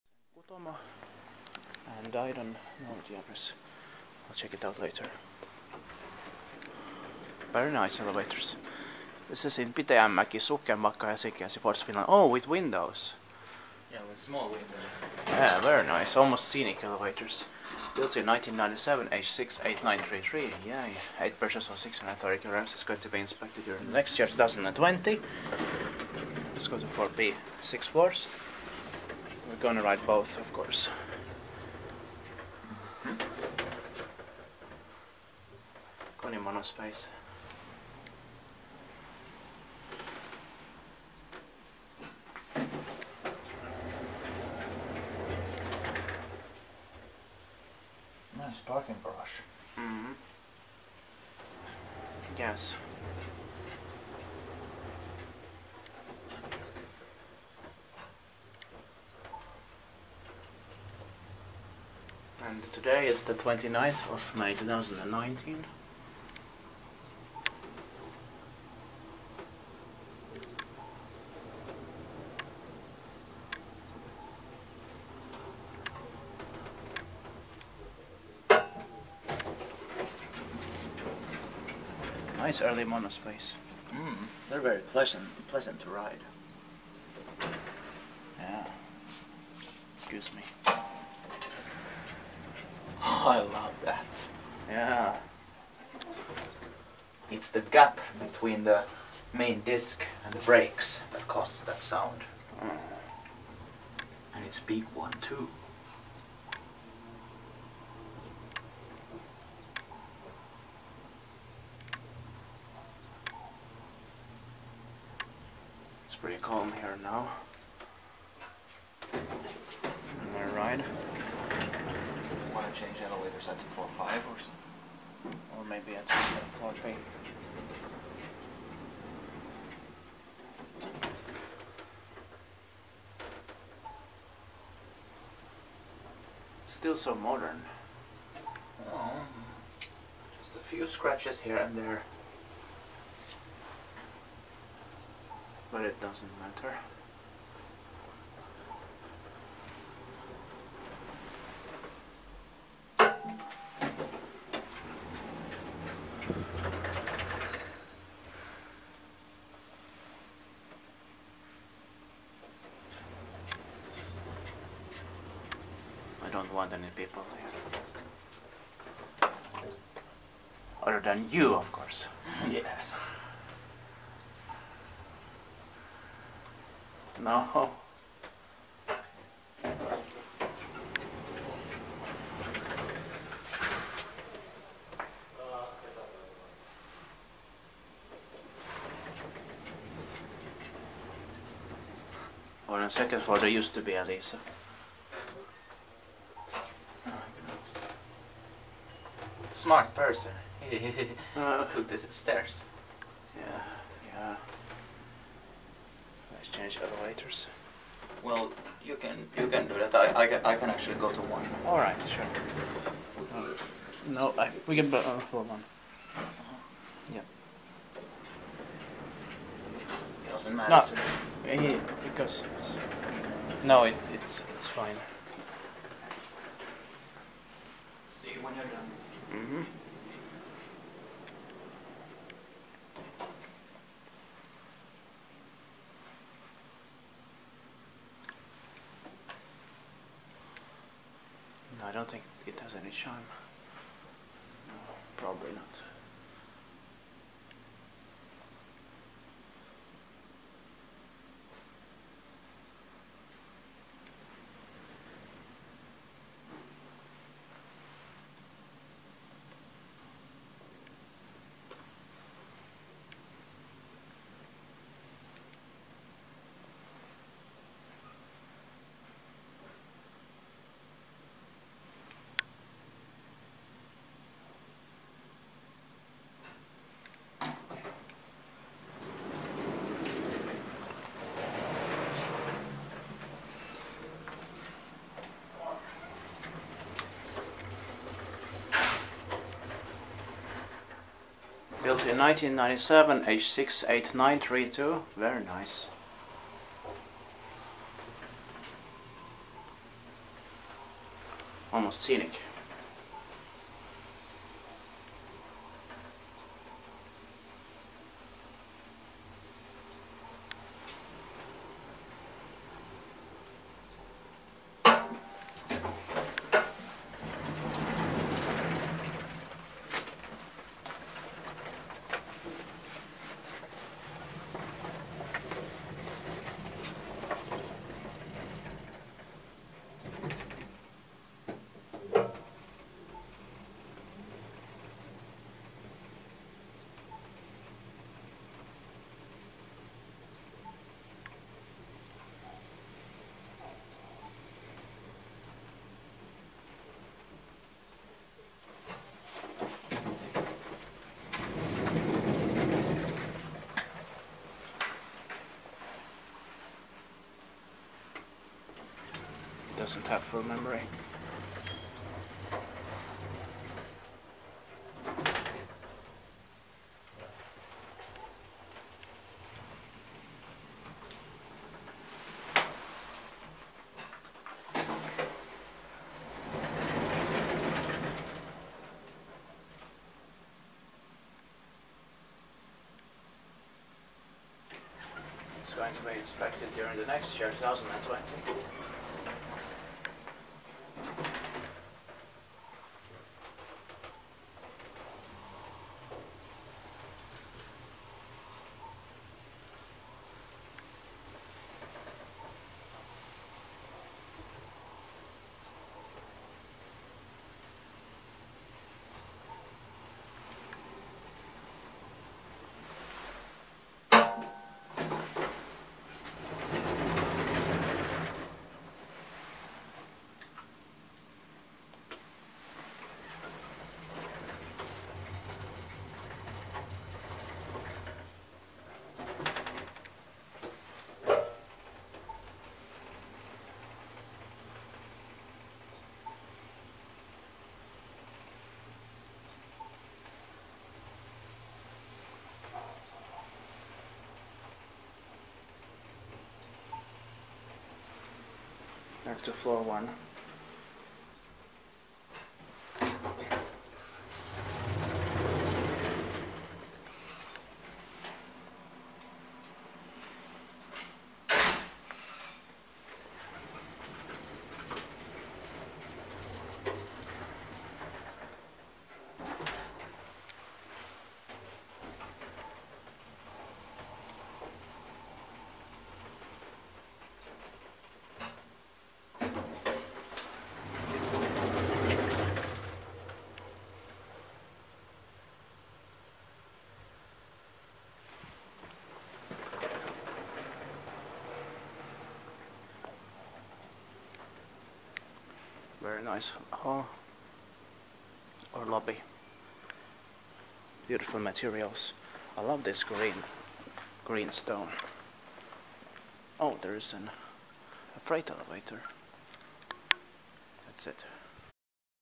Very Nice 1997 KONE Monospace MRL Tr. Elevators with windows@Kutomotie 18, Pitäjänmäki, Helsinki, Fi
Location: Kutomotie 18, Pitäjänmäki / Sockenbacka, Helsinki / Helsingfors, Finland

Brand: KONE
Type: KONE Monospace MRL Traction, Ecodisc

Year installed: 1997

Floors served: 6 (P, *1*, 2, 3, 4, 5)
Serial: H 68932 & H 68933
Capacity: 8 persons or 630 kg